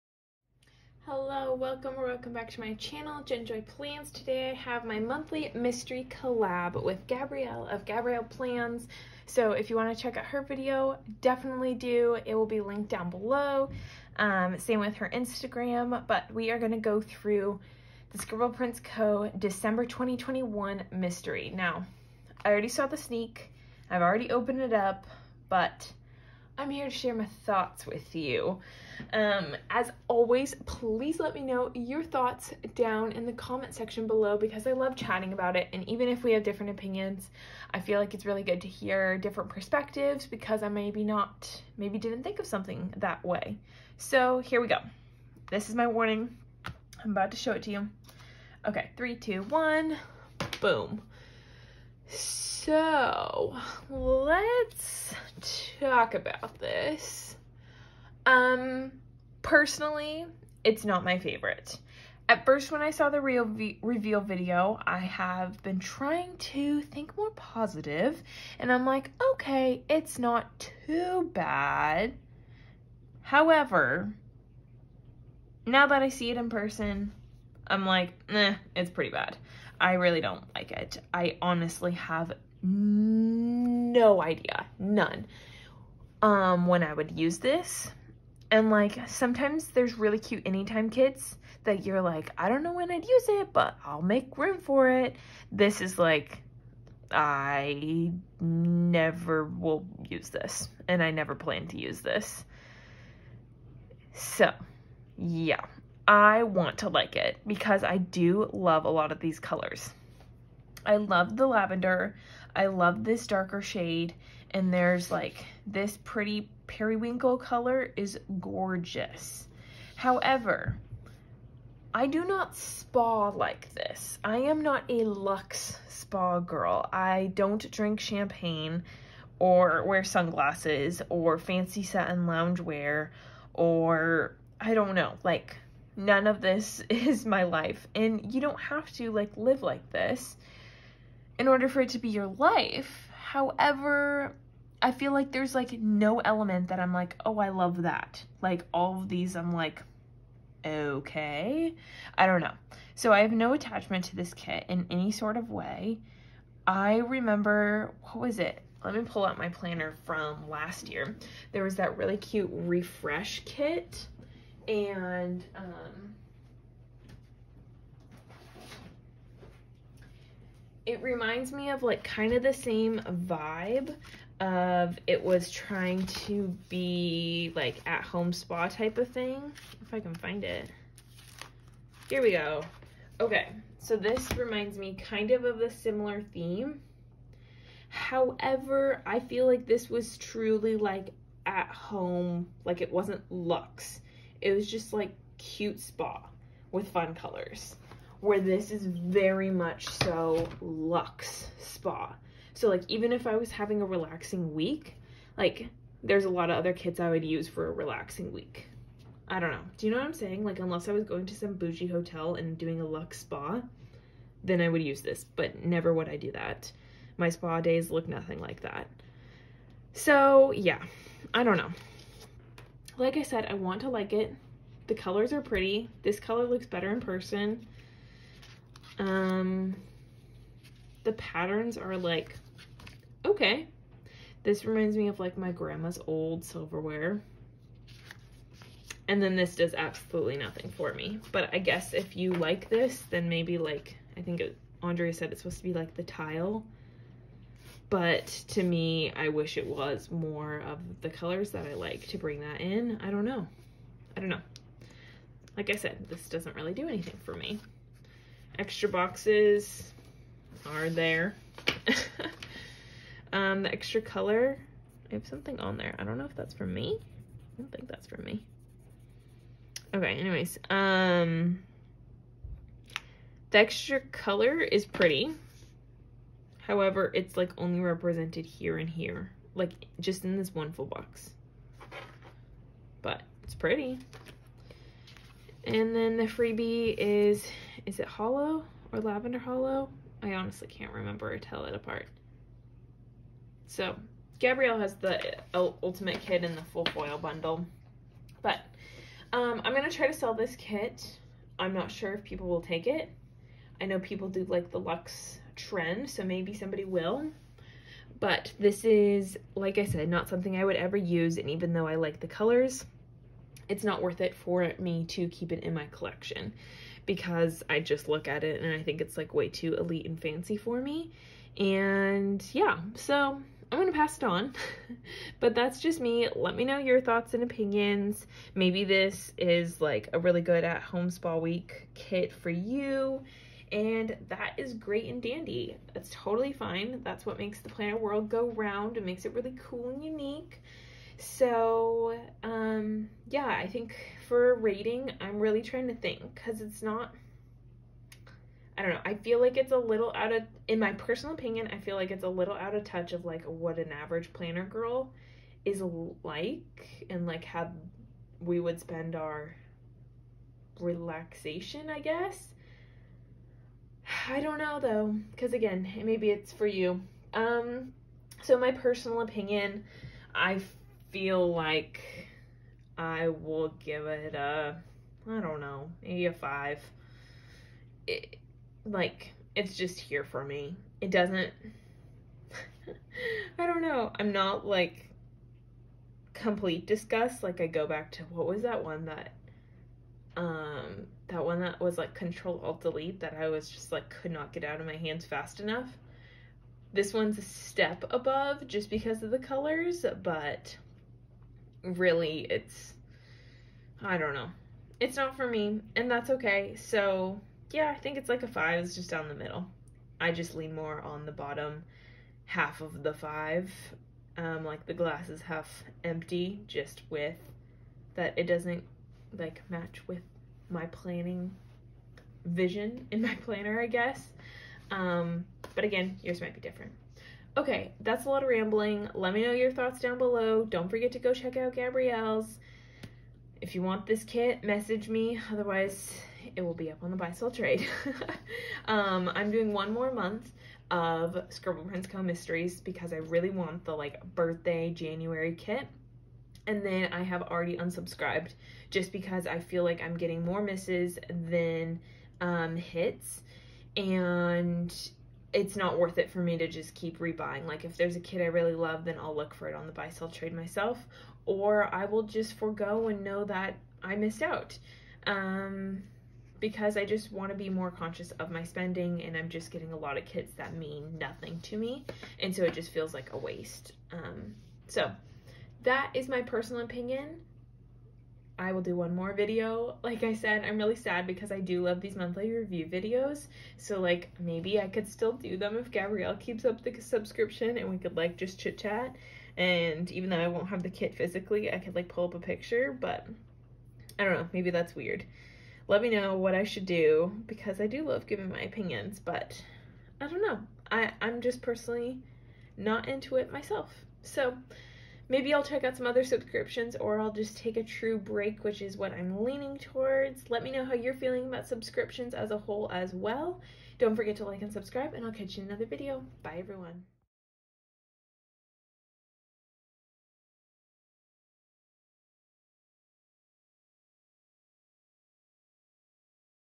SPC December 2021 Mystery Kit Reaction *SPOILER* // Collab with GabriellePlans 💜
Let me know your thoughts on this months mystery and if/when you are using it!!

Make sure to check out Gabrielle's Video // Also, check Gabrielle out on Instagram @gabrielleplans!!

Check me out on instagram // jenjoyplans

Stickers featured in this video:
//KIT//
December 2021 Mystery